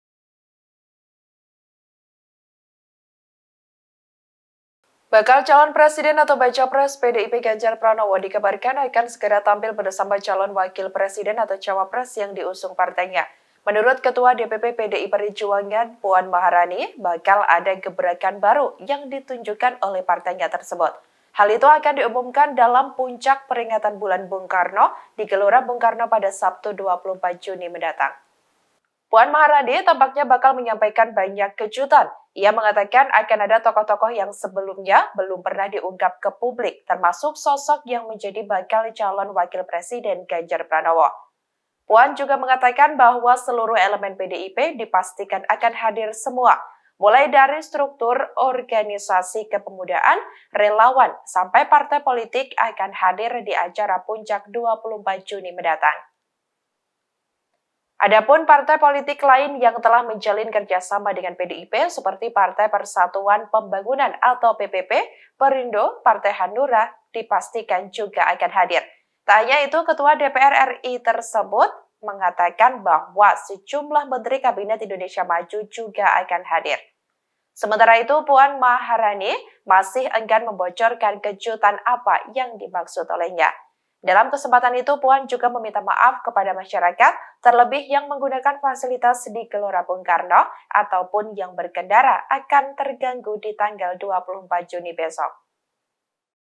Puncak Bulan Bung Karno, PDI P Bakal Banyak Kejutan, Termasuk Munculkan Bacawapres Ganjar
TRIBUNSOLO.COM - Bakal calon presiden (Bacapres) PDI-P, Ganjar Pranowo dikabarkan akan segera tampil bersama calon wakil presiden (cawapres) yang diusung partainya.

Menurut Ketua DPP PDI Perjuangan, Puan Maharani, bakal ada gebrakan baru yang ditunjukkan oleh partainya.

Hal itu akan diumumkan dalam puncak peringatan Bulan Bung Karno di Gelora Bung Karno pada Sabtu (24/6) besok.

Dilansir dari Tribunnews.com pada Jumat (23/6), Puan Maharani tampaknya bakal menyampaikan banyak kejutan.

Ia mengatakan, akan ada tokoh-tokoh yang sebelumnya belum pernah diungkap ke publik termasuk sosok yang menjadi bakal cawapres Ganjar.

"Bisa saja dari orang-orang yang kemudian yang nanti hadir merupakan bacawapres-bacawapres dari bacapres PDI Perjuangan yang belum pernah saya sebutkan, atau mungkin belum kelihatan," ujar Puan.

Puan juga mengatakan bahwa seluruh elemen PDI-P dipastikan akan hadir semunya.

Mulai dari struktur, organisasi kepemudaan, relawan sampai partai politik akan hadir di acara puncak 24 Juni, mendatang.

“Tentu saja seluruh struktural dari seluruh Indonesia plus partai-partai politik, ketua umum partai politik, juga sahabat-sahabat PDI Perjuangan, teman-teman PDI Perjuangan dan orang-orang yang kemudian merasa mempunyai pandangan, cita-cita sama dengan PDI Perjuangan semuanya Insyaallah kita undang,” kata Puan lagi.

Adapun, partai politik lain yang telah menjalin kerja sama dengan PDI-P seperti Partai Persatuan Pembangunan (PPP), Perindo, Partai Hanura dipastikan akan hadir.

“Yang lain-lain kami masih dalam konfirmasi, namun Insyaallah besok sudah ada kepastian pihak yang bersangkutan akan hadir,” ungkap Puan.

Tak hanya itu, Ketua DPR RI ini menyebut bahwa sejumlah Menteri Kabinet Indonesia Maju juga diundangan ke acara itu.

“Sebagian menteri dari kabinet kita undang, karena sebagian lagi kita konfirmasi ternyata yang bersangkutan sedang berdinas ke luar kota atau luar negeri. Mungkin ada yang hadir, mungkin ada yang tidak,” sambung dia.

Sementara itu, Puan masih enggan membocorkan kejuatan apa yang dimaksud.

“Ya biasanya PDI Perjuangan itu kalau bikin kejutan itu enggak pakai tanda-tanda tapi mendadak, namanya kejutan ya begitu. Jadi semoga tanggal 24 akan ada kejutan-kejutan yang indah, menggembirakan dan membuat kita semua merasa 'Wah gitu ya PDI Perjuangan',” ungkap Puan.

Dalam kesempatan itu, Puan juga meminta maaf kepada masyarakat.

Terlebih yang menggunakan fasilitas di GBK atau pun yang berkendara akan terganggu di tanggal 24 Juni.

“Saya juga mungkin harus mohon maaf kepada seluruh orang-orang yang selama ini sering menggunakan fasilitas GBK untuk olah raga, untuk kegiatan lainnya mungkin besok tanggal 24 akan terganggu kegiatan rutinnya,” ujar Puan.

(Tribun-Video.com/Tribunnews.com)

Artikel ini telah tayang dengan judul 'Puan Maharani Sebut Akan Ada Kejutan di Puncak Bulan Bung Karno, Cawapres Ganjar Bakal Dimunculkan?'.